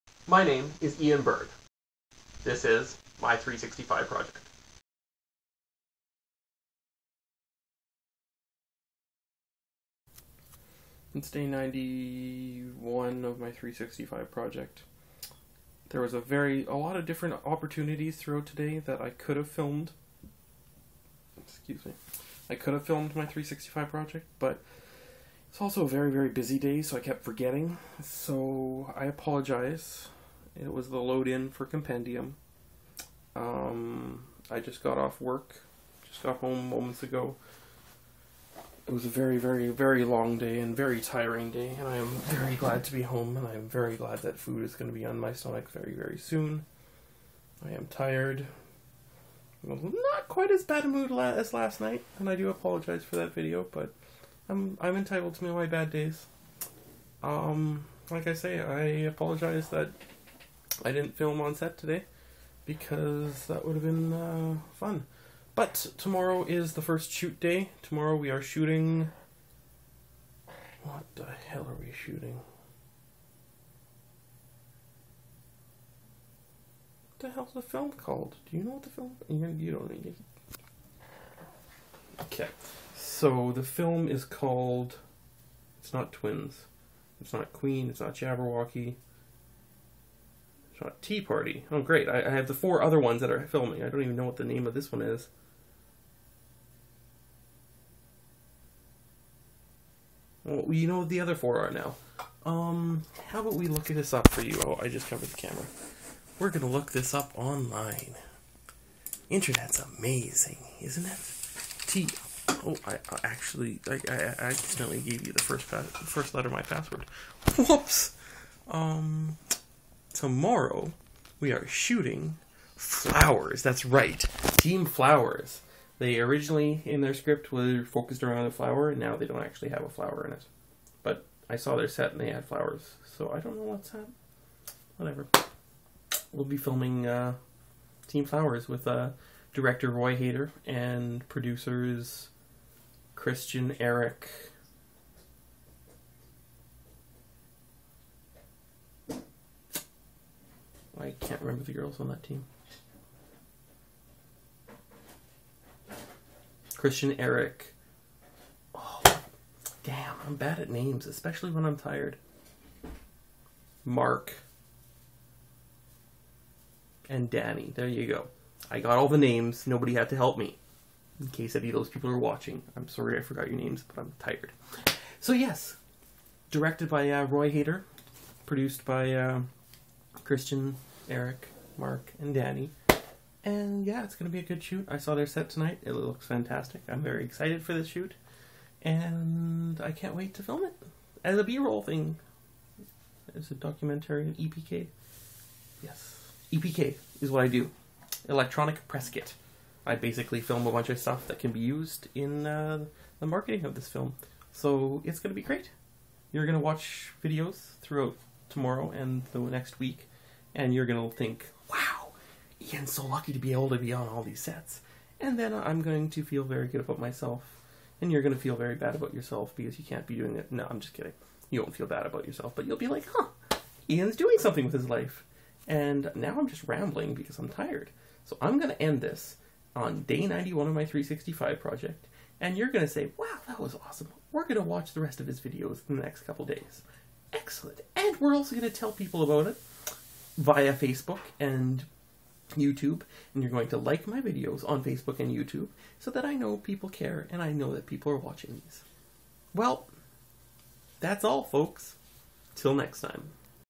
Day 91 of my 365project - Compendium begins!
Load in for Compendium day. ending with me being very tired and rambling like a crazy man.